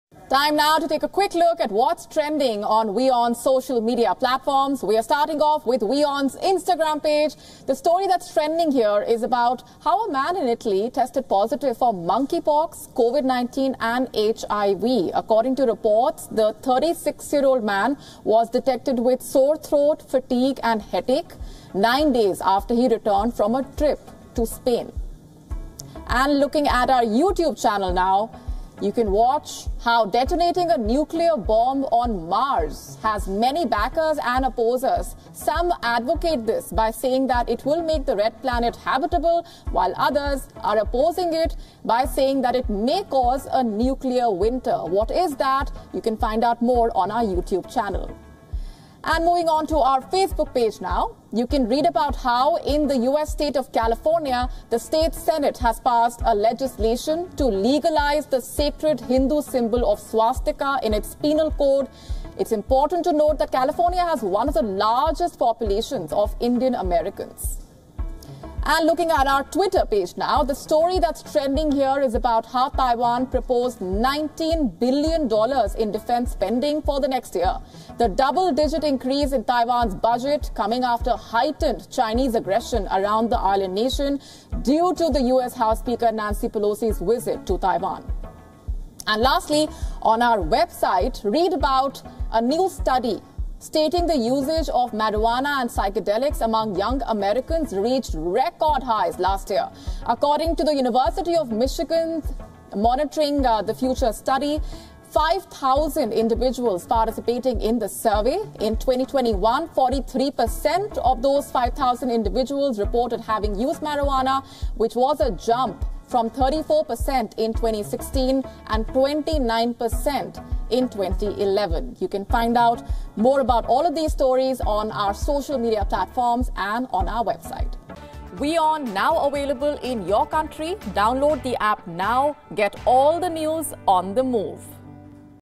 Trending on WION: Italian man detected with Covid-19, Monkeypox and HIV at the same time
Let us take a look at stories generating interest across WION's social media channels.